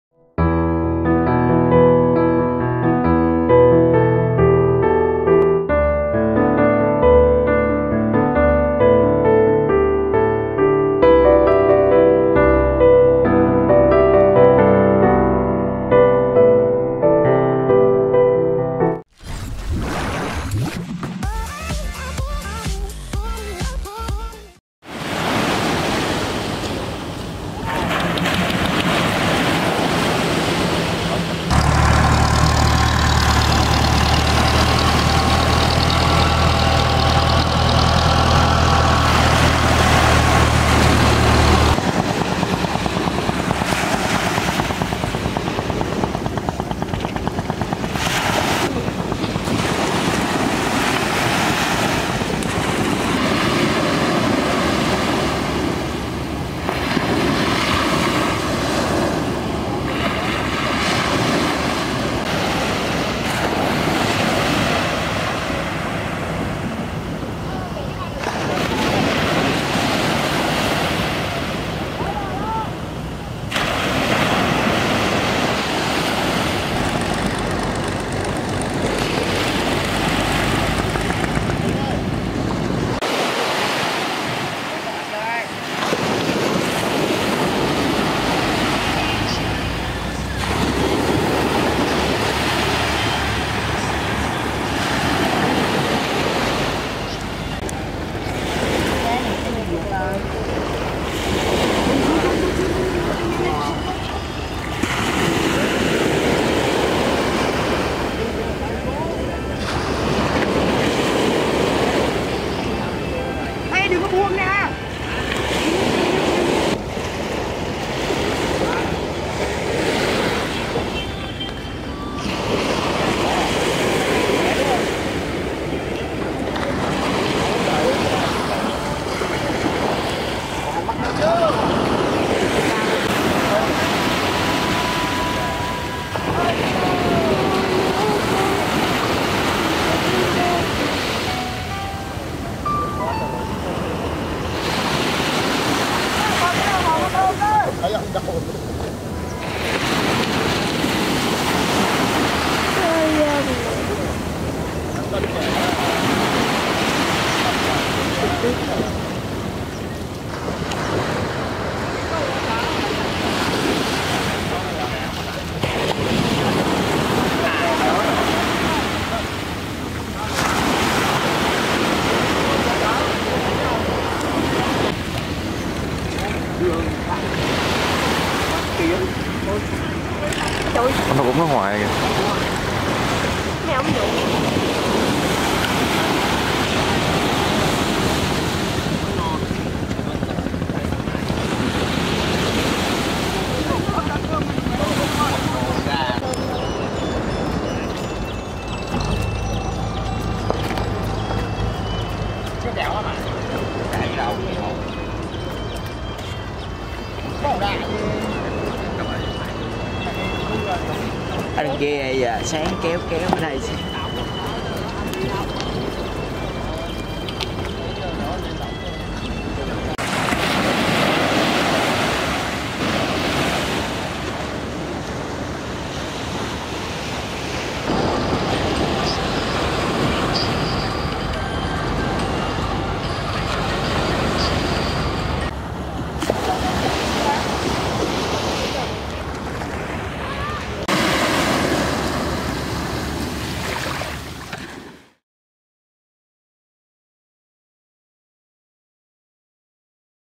VUNG TAU - Sunrise l time lapses HD l
It's simply a peaceful morning on the beach.
© Bản quyền thuộc về  Nghia Pham Bizi
© Copyright by Nghia Pham V84 ☞ Do not Reup